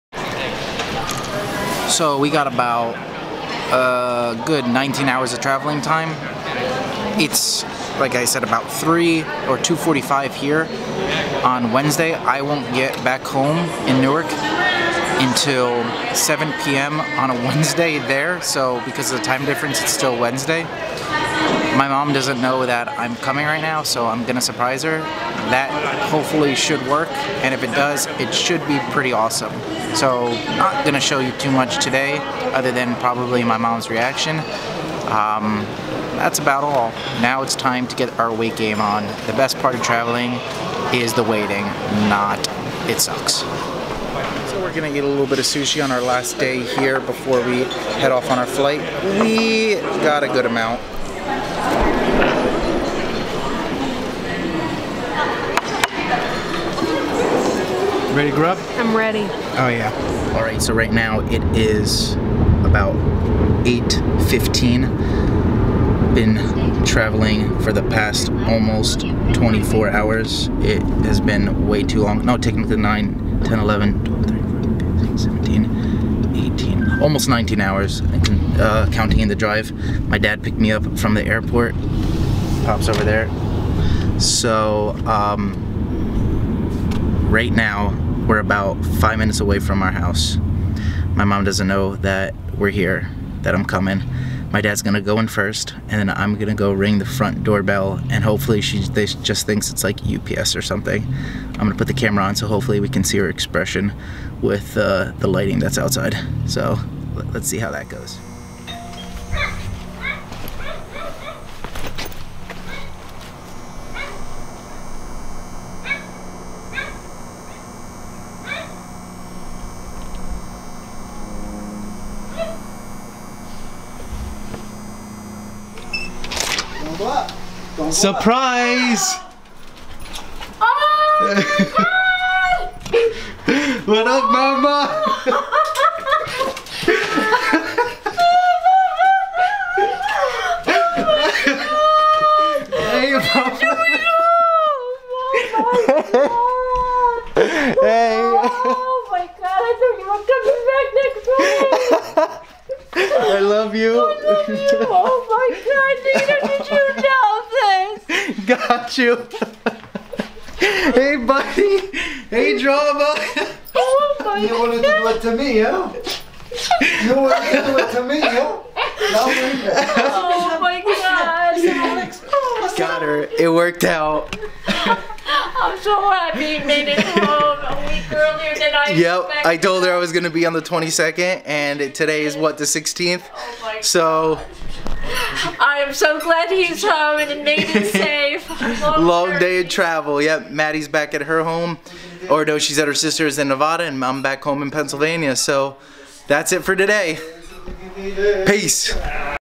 Surprised my mom! Back home from peace corps service!
Told my mama I was coming home a week later. Well I lied!